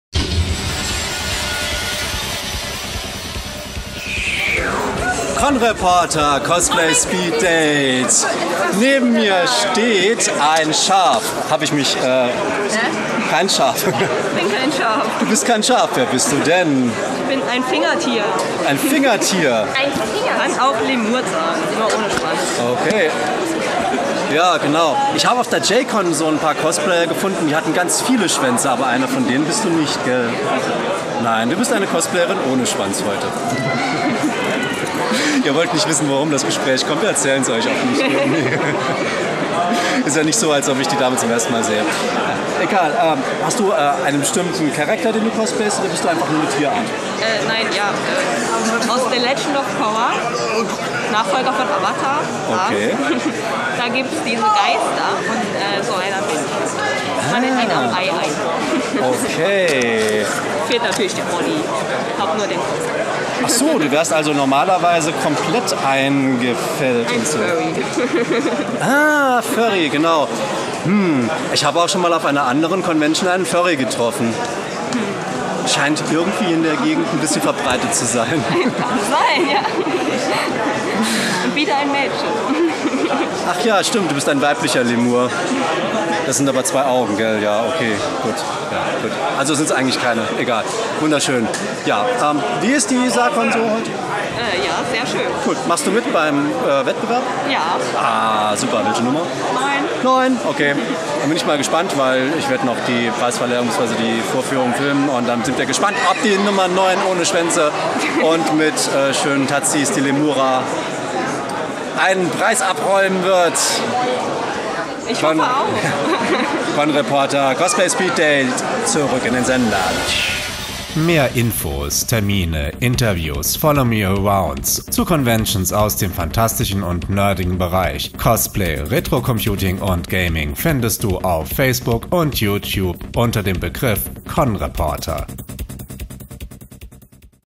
Lemur [ Avatar ] Unknown Cosplay Girl @ SAARCON 2015
COSPLAY SPEED DATE ! Was ist das ? Ein Schaf ? Ach nein, ein Fingertier, ein Lemur aus Avatar Herr der Elemente ! Hätte ich ohne ihre neun Schwänze fast gar nicht wiedererkannt „smile“-Emoticon Eine der wenigen Furry Cosplayerinnen im Saarland, zwei Daumen auf HOCH